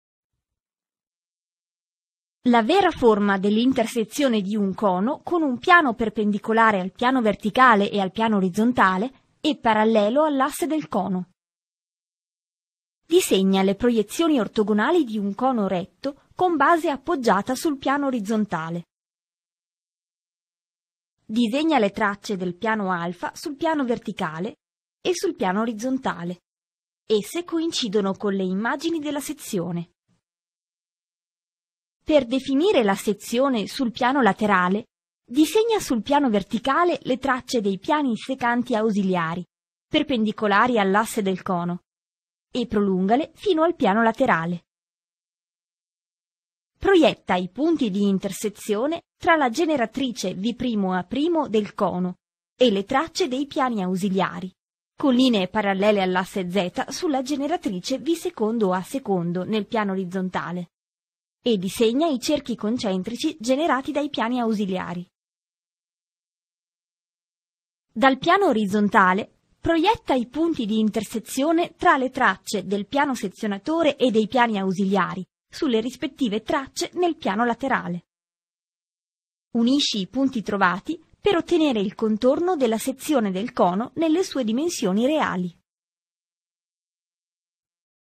Vera forma dell'intersezione di un cono con un piano perpendicolare al PV e al PO
Un breve filmato su come disegnare la vera forma dell'intersezione di un cono con un piano perpendicolare al PV e al PO e parallelo all'asse del cono. Per approfondire l'argomento, visita il sito: